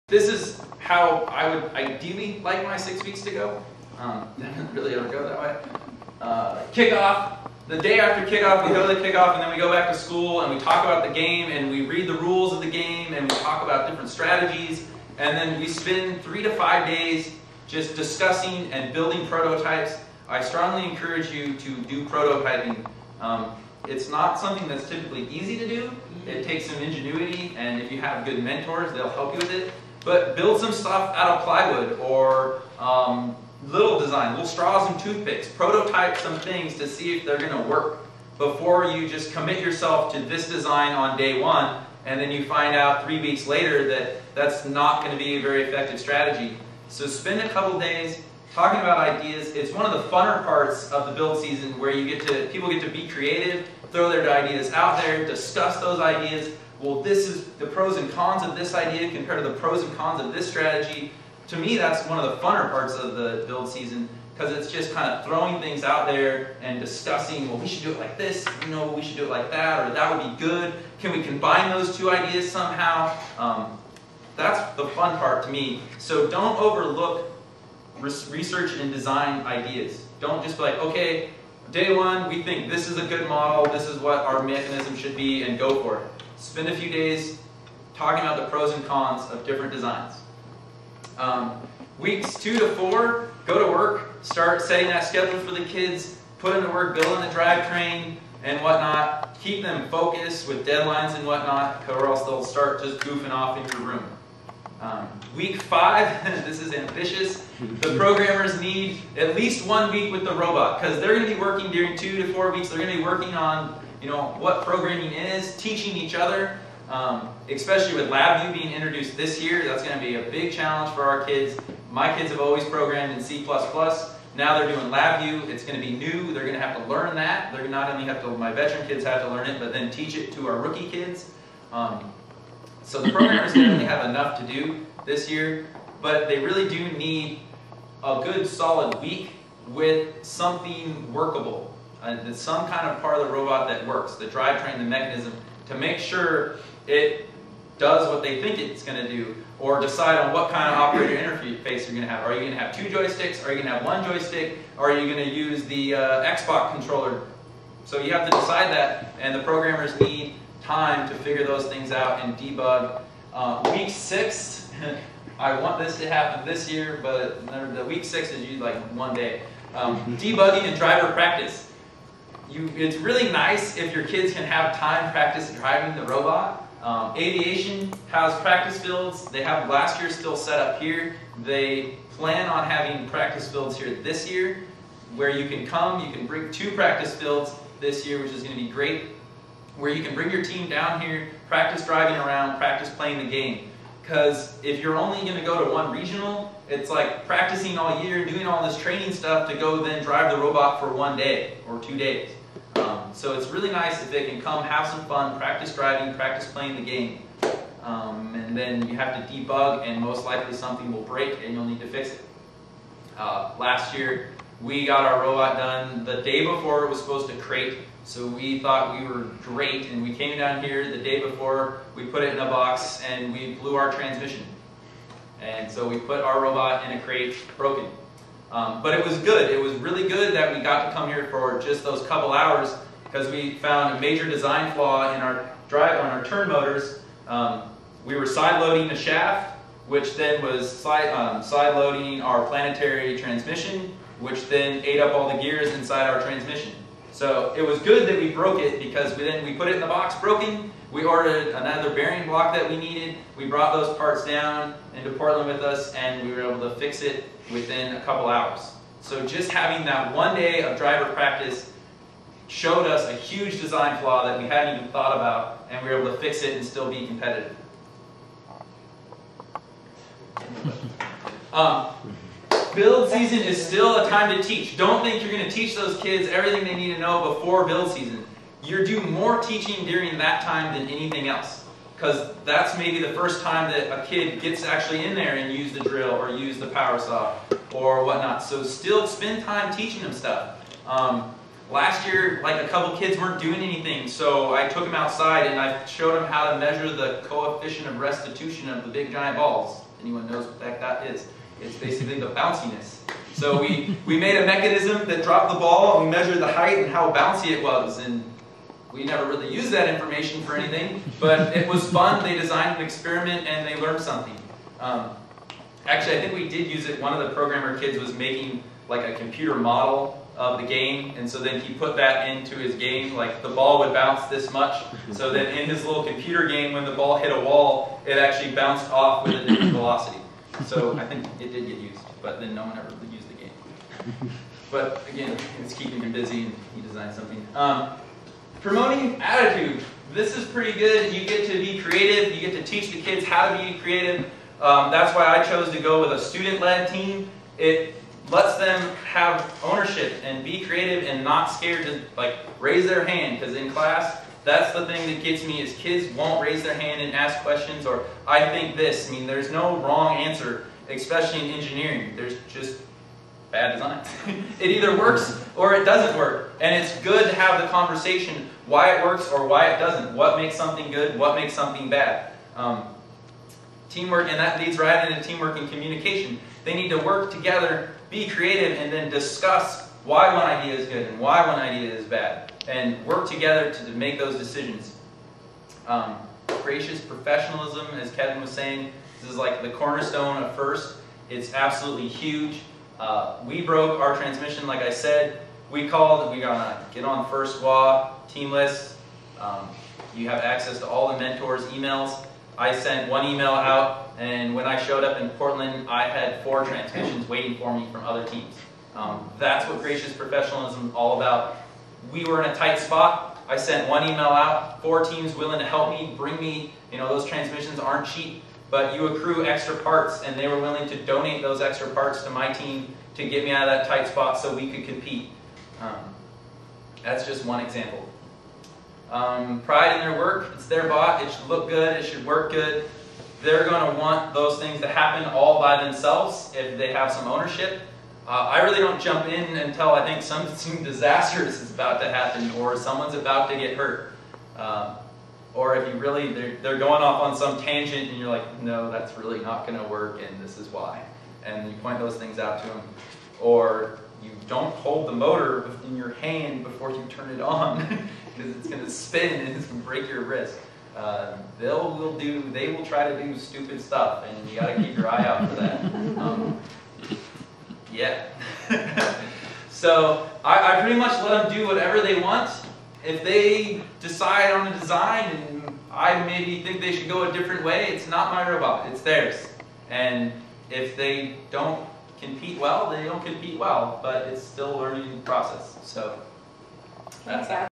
PNW FIRST Rookie Workshops - Being A Mentor Part 5
Being A Mentor with Jared Fernandez, Part 5/7
PNW FIRST Oct 18th Rookie Workshops at Aviation HS